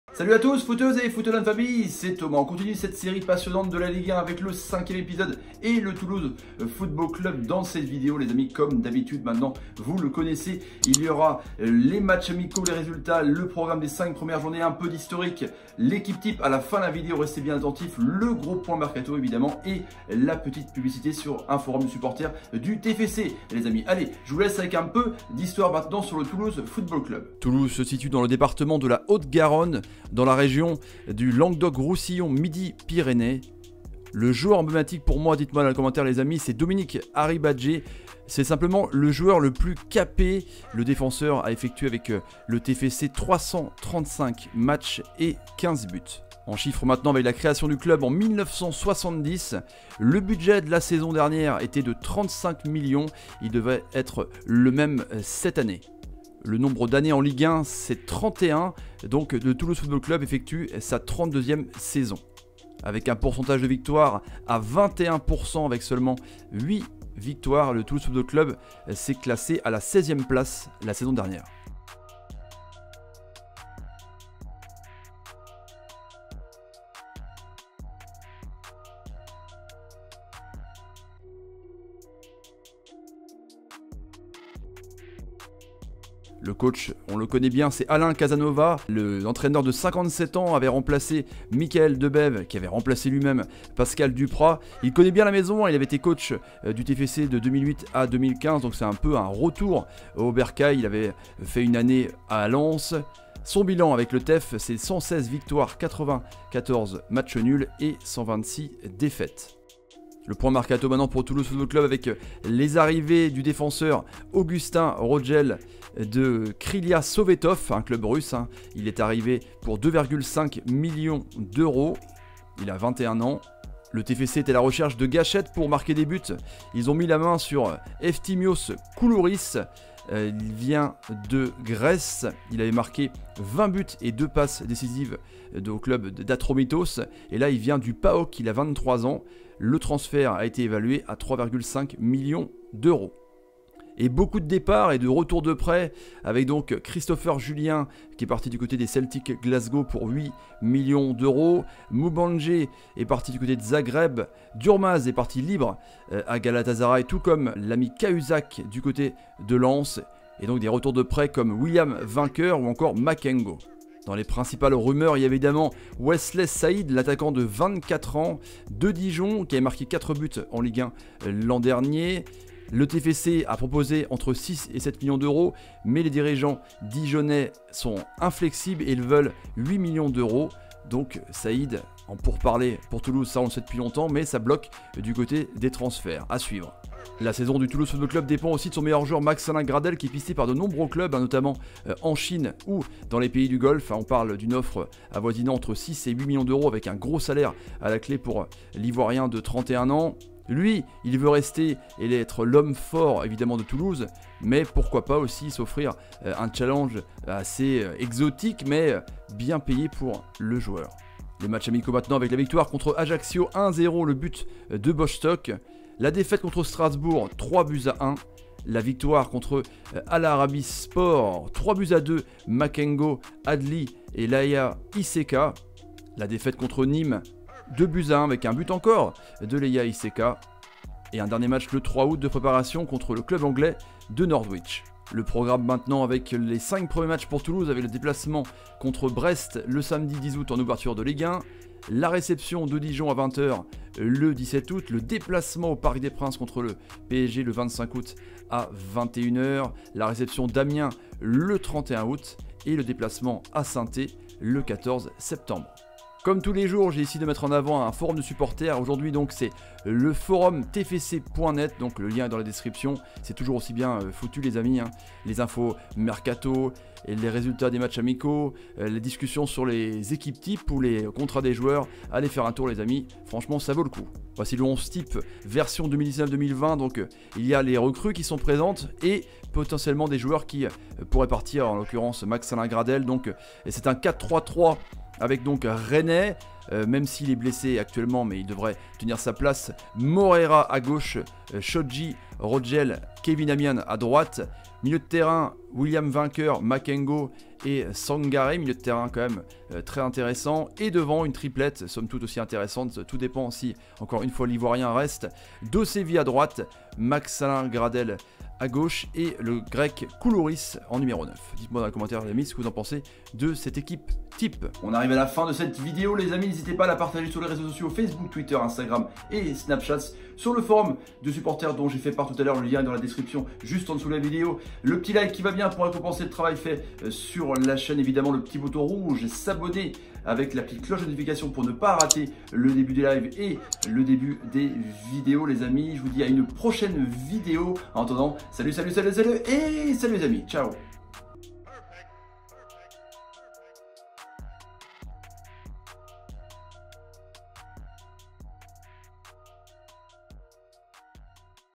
Présentation TOULOUSE FC Ligue 1 | Saison 2019-2020 / Mercato du Club
Voici la présentation complète en vidéo du Toulouse FC pour la Saison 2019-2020 dans cette Ligue 1

Nouvelle série de vidéos pour présenter cette Ligue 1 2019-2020. Au programme : historique, coach, un gros point mercato, les résultats des matchs amicaux, l'équipe-type, les premières recontres du championnat et focus sur un forum de supporters. Place au 16 ème de Ligue 1 l'an dernier : Le TFC